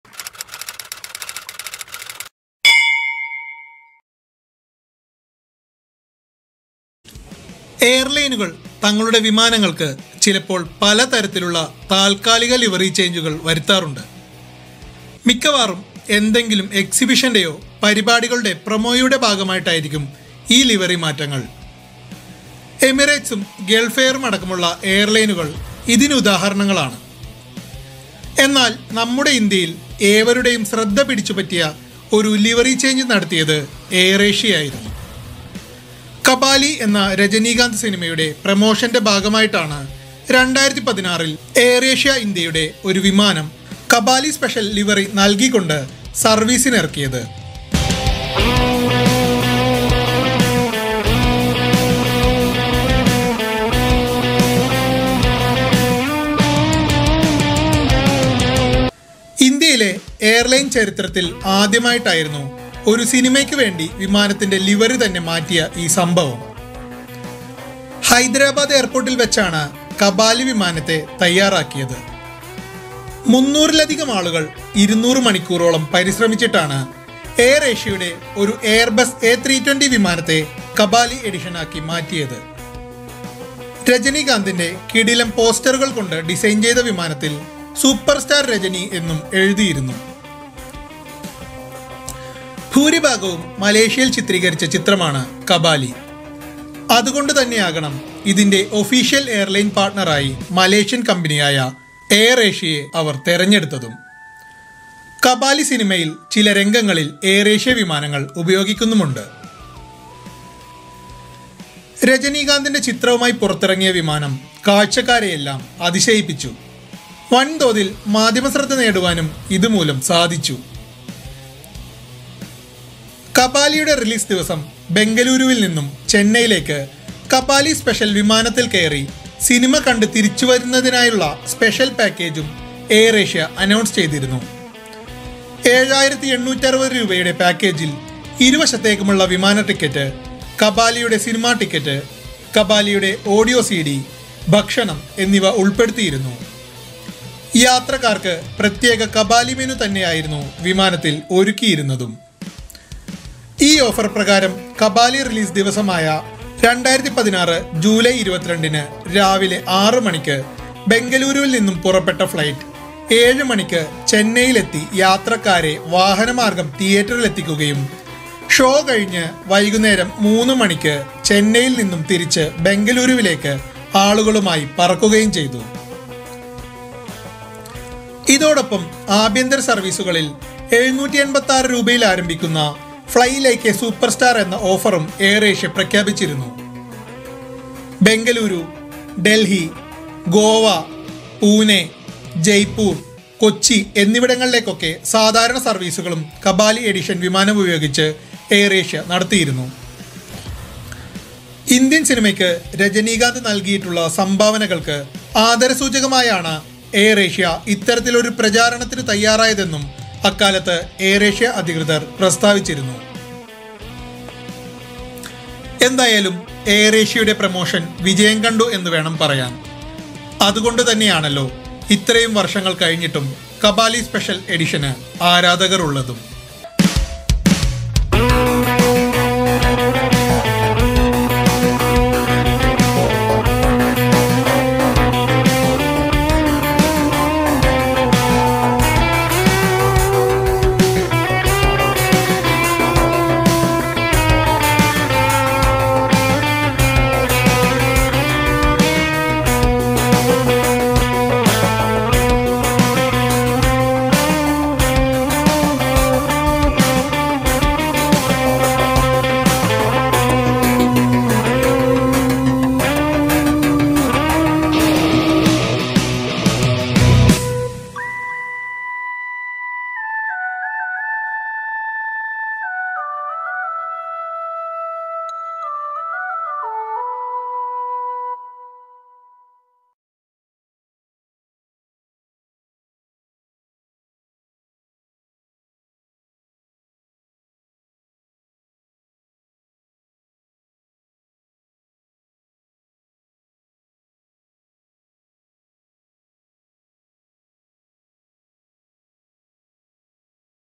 Air Asia Kabali Edition Airbus A320 Flight | എയർ ഏഷ്യയുടെ 'കബാലി' രജനി സ്പെഷ്യൽ എഡിഷൻ വിമാനം
കബാലി എന്ന രജനീകാന്ത് സിനിമയുടെ പ്രൊമോഷന്റെ ഭാഗമായിട്ടാണ് 2016 ൽ എയർ ഏഷ്യ ഇന്ത്യയുടെ ഒരു വിമാനം കബാലി സ്പെഷ്യൽ ലിവെറി നൽകി സർവ്വീസിനിറക്കിയത്. ഇന്ത്യയിലെ എയർലൈൻ ചരിത്രത്തിൽ ആദ്യമായിട്ടായിരുന്നു ഒരു സിനിമയ്ക്ക് വേണ്ടി വിമാനത്തിന്റെ ലിവെറി തന്നെ മാറ്റിയ ഈ സംഭവം.

AirAsia India, the official airline partner for Rajinikanth-starrer Kabali, has devoted a special aircraft, bearing the images of the star, to the movie and the fans of the Tamil superstar.

As an official airline partner for Kabali, it has also planned a special flight from Bengaluru to Chennai for fans to watch the first-day-first-show of the movie.

A massive tribute to Superstar Rajinikanth from the AirAsia Allstars family.
One Man, One Airline.